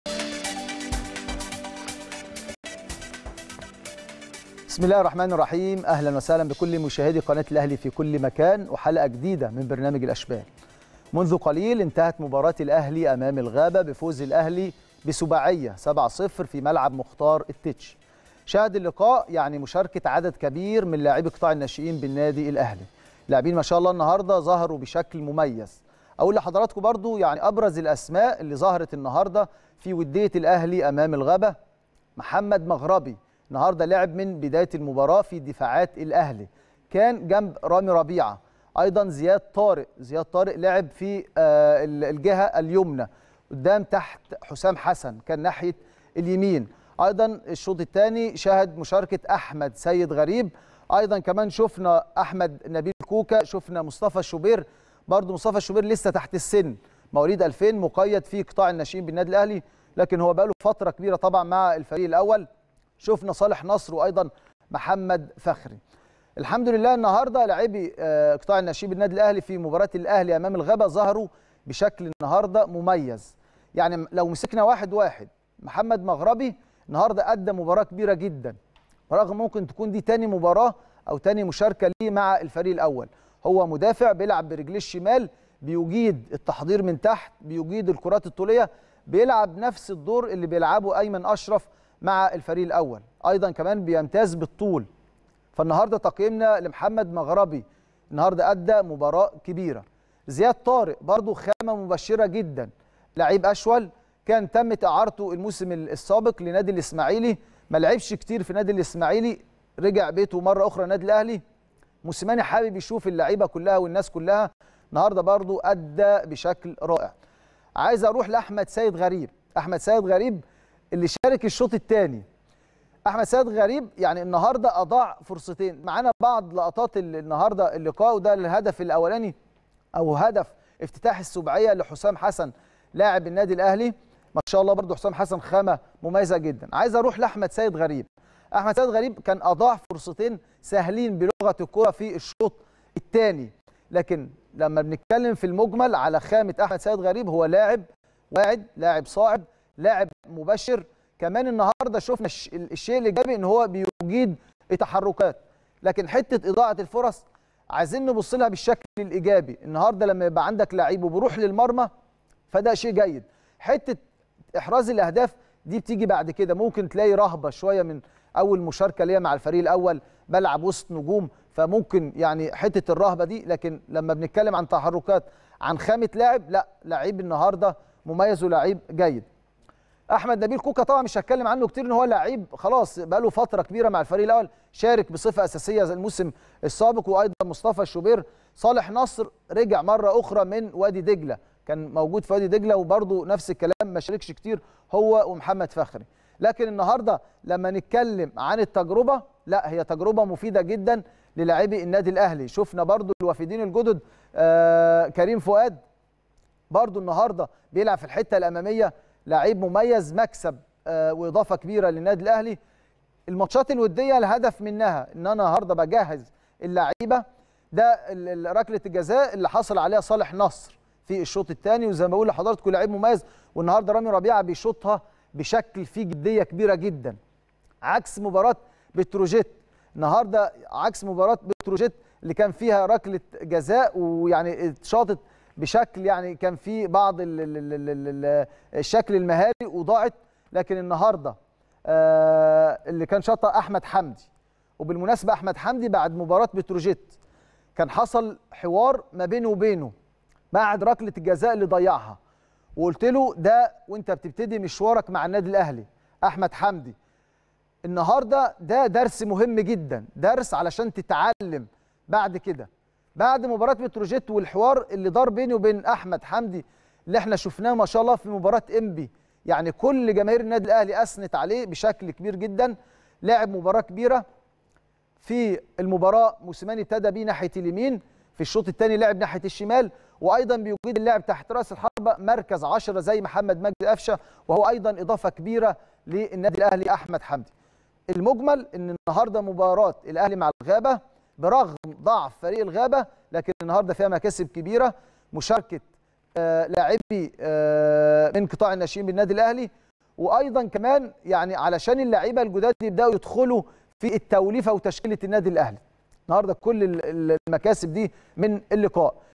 اسامة حسني يبرز تألق الناشئين أمام الغابة ويشيد بمستواهم ويلقي الضوء على المكاسب ودية اليوم.
اسامة حسني يبرز تألق الناشئين أمام الغابة ويشيد بمستواهم ويلقي الضوء على المكاسب ودية اليوم | الأشبال